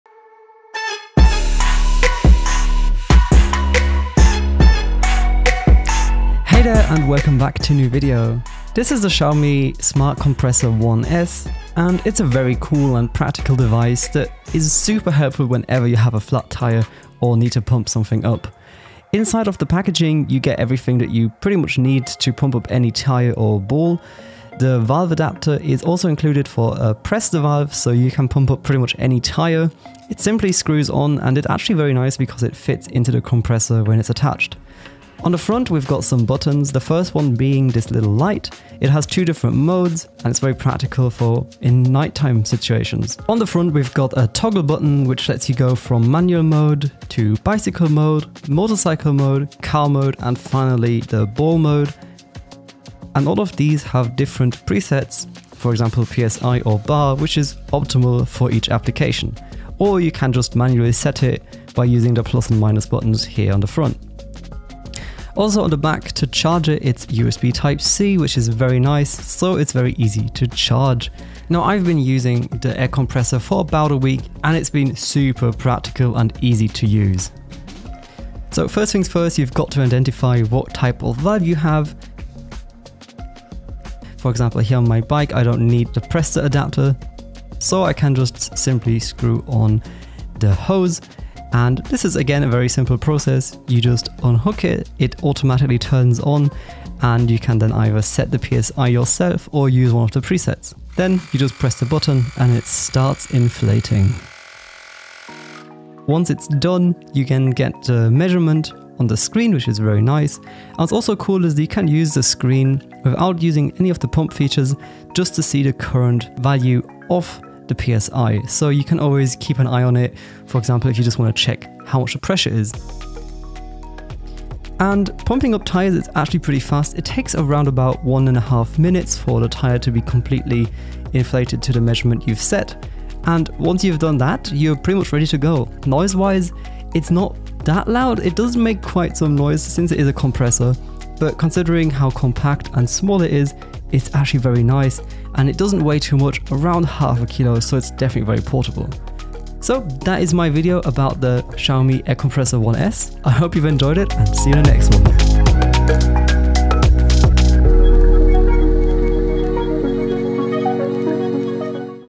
Xiaomi Air Compressor 1S — Test and Features
The Xiaomi Air Compressor 1S is a portable and practical electric air pump, that can quickly inflate any tire as it includes modes to pump up bikes, motorbikes, electric scooters and car tires, as well as balls.

Included in the box, you'll get a presta valve adaptor and needle bit. It also comes with a nice carrying pouch for keeping everything organised. It's super practical when you need to pump up anything from city bikes, mountainbikes, car tires, electric scooters, footballs, and more.

Chapters
00:00 Intro and overview
00:34 Built-in flashlight
00:43 Modes
01:04 USB-C Port
01:11 Usage and testing
01:51 See current tire pressure
02:05 Speed, noise and weight
02:29 Outro